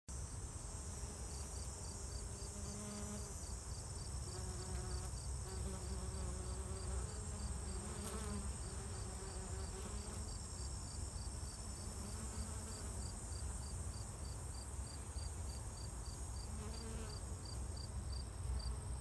Bees on goldenrod
Goldenrod hums with insect activity